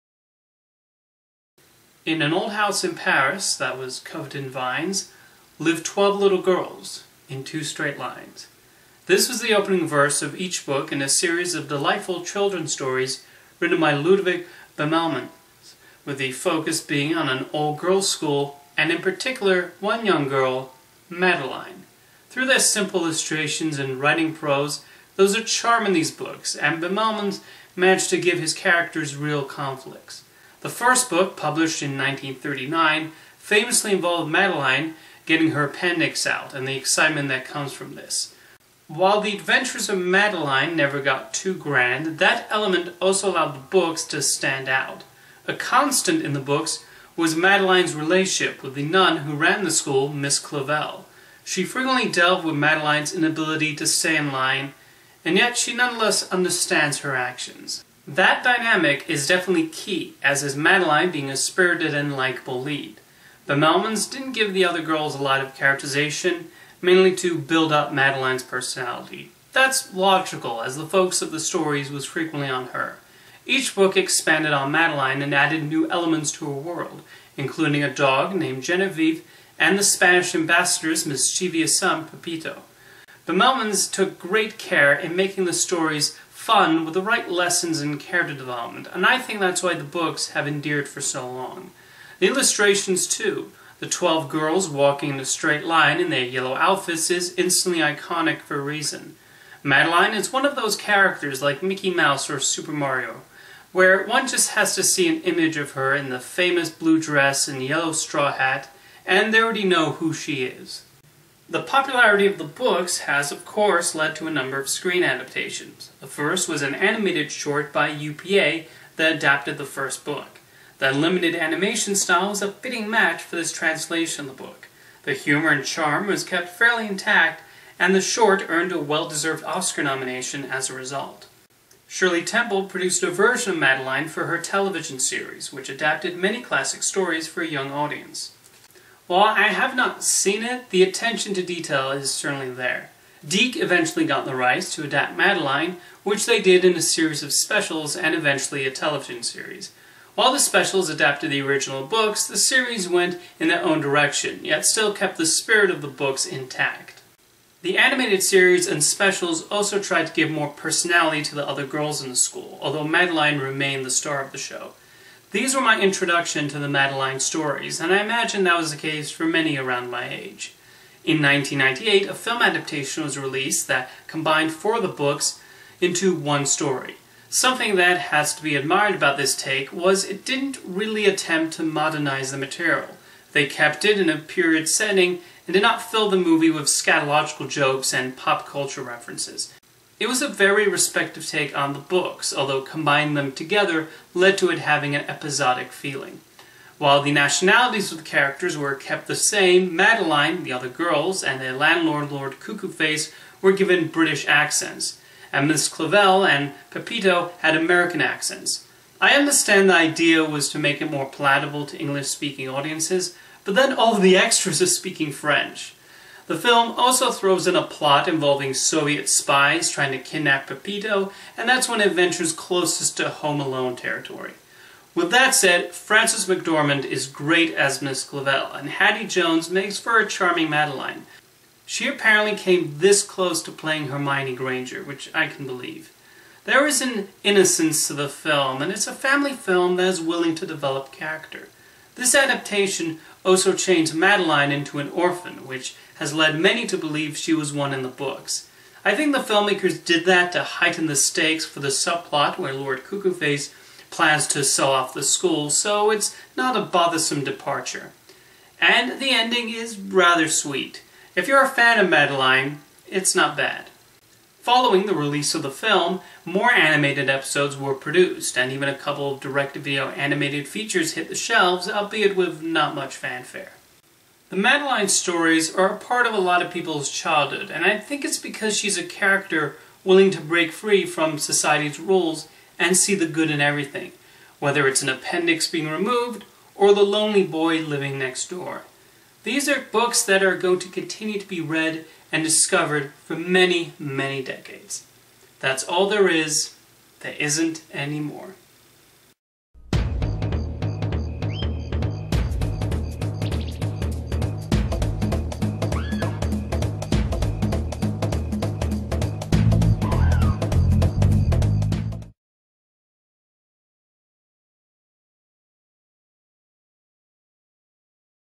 Character Profile: Madeline
I see why Ludwig Bemelmans's books about a French girl living in an old house covered in vines continue to charm on the page and the screen.